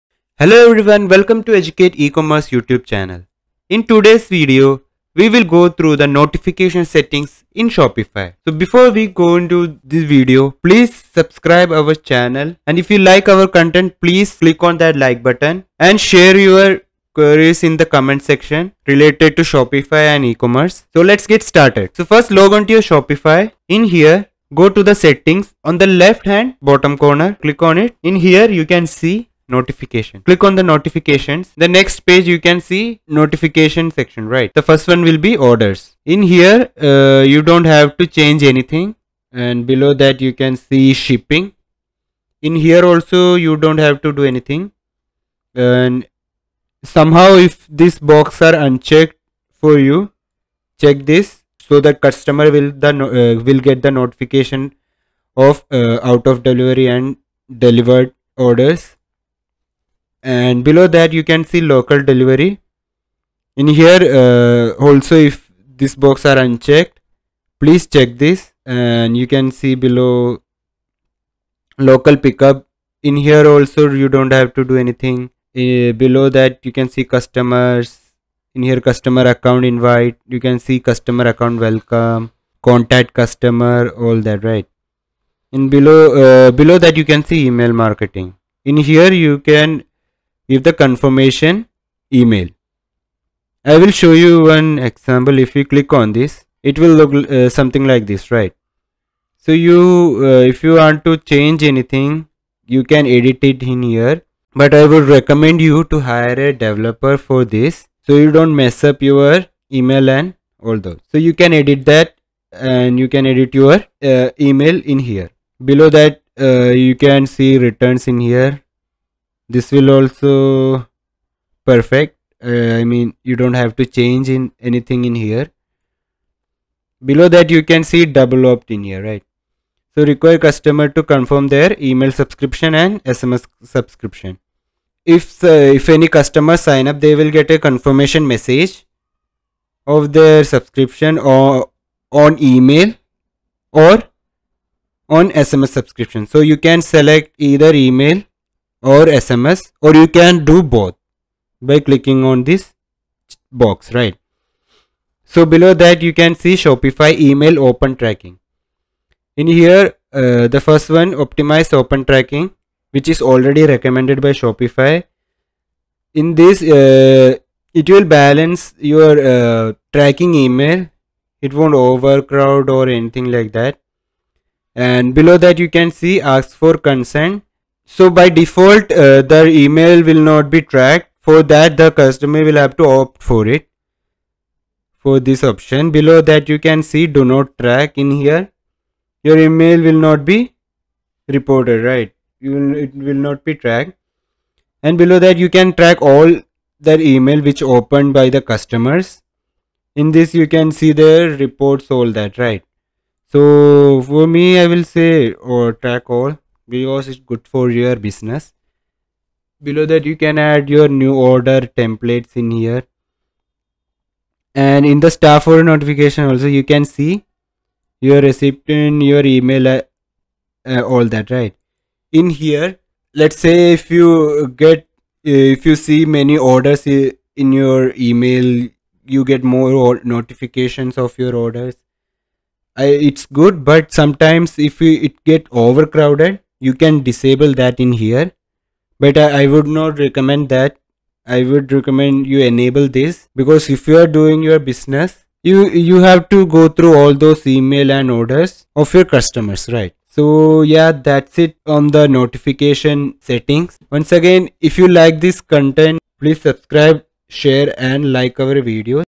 Notification Settings in Shopify | Educate E commerce
Your store comes with email templates that you can customize with your logo and brand accent color. You can choose to have email notifications sent to you, your staff, or your customers, depending on the setting for your order notifications.

Your store also includes SMS notifications for order confirmation and shipping updates. To learn more about how to enable and configure these updates, see SMS notifications.

You can view and update your store's notification settings from the Notifications settings page in your Shopify admin.

  00:00 - Introduction
  00:21 - Notification Settings
  04:10 - End

Shopify Store setup made easier with Educate E-Commerce Tutorials.